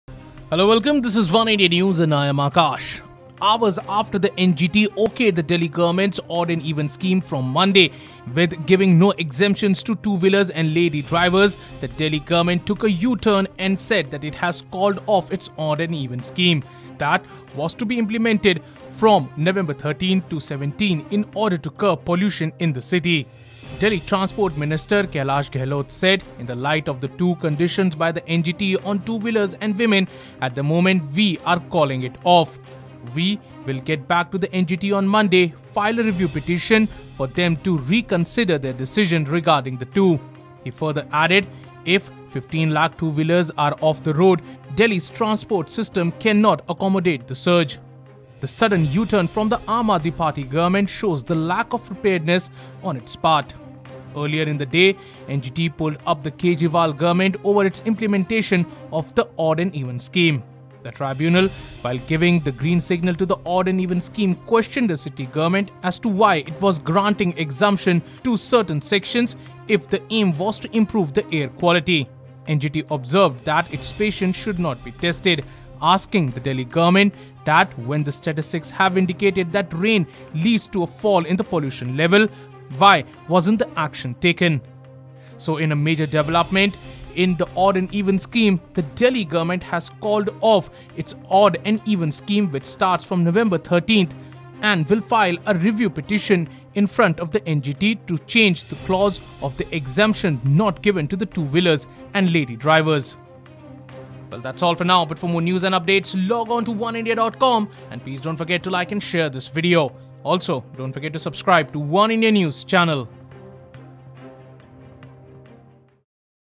Delhi Air pollution: Delhi government calls off Odd-Even after NGT directs changes | Oneindia News
Hour after the NGT directs new changes to the Delhi government's odd and even scheme from monday, exempting two wheelers and lady drivers, the Delhi government took an U-turn and has called off its odd-even scheme, that was to be implemented from nov 13 to 17 in order to curb pollution in the city. Delhi transport minister Kailash Gehlot said “In light of the the two conditions by NGT on 2 wheelers & women, at the moment we are calling it off. We will go back to NGT on Monday, file a review petition for them to reconsider their decision regarding the two”. He futher added “"If 30 lakh two-wheelers are off the road, Delhi's public transport system cannot accommodate the surge,". The sudden u-turn from the AAP government shows the lack of preparedness on its part. Earlier in the day NGT pulled up the Kejriwal government over its implementation of the odd-even scheme. The tribunal, while giving the green signal to the odd-even scheme, questioned the city government as to why it was granting exemptions to certain sections if the aim was to improve air quality.